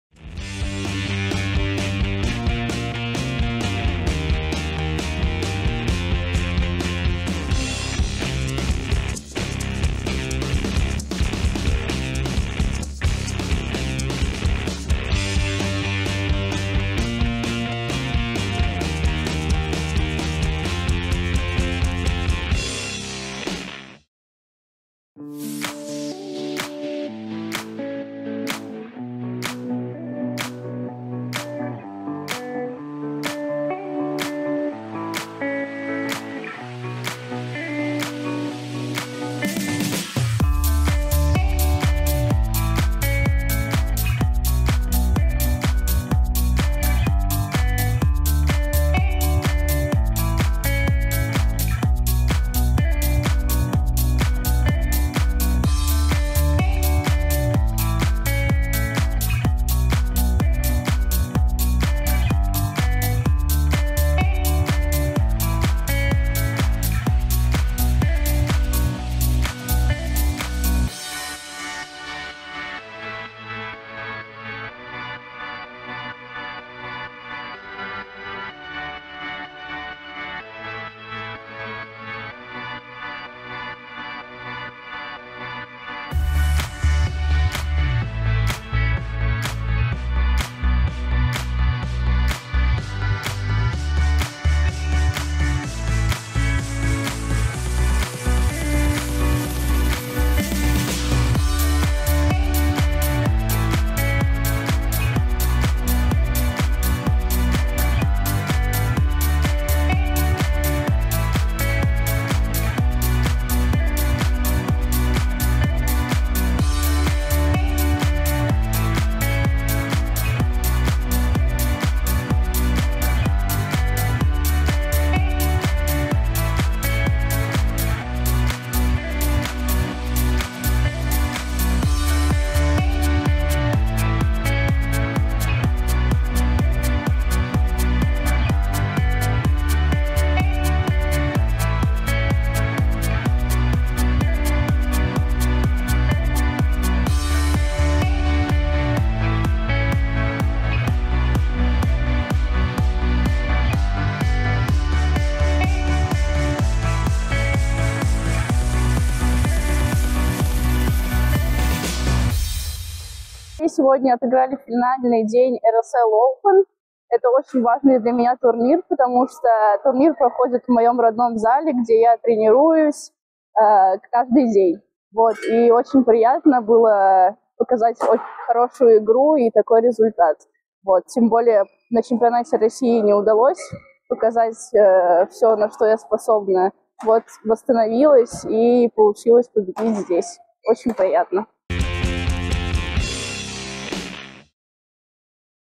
Анастасия Алимова на всероссийских соревнованиях по бадминтону «RSL OPEN 2022».
Анастасия Алимова стала победителем всероссийских соревнований по бадминтону «RSL OPEN 2022».

Анастасия завоевала «золото» в одиночном разряде. В финале она встретилась с Александрой Чушкиной. Игру сыграли в две партии со счетом 21/13 и 21/17.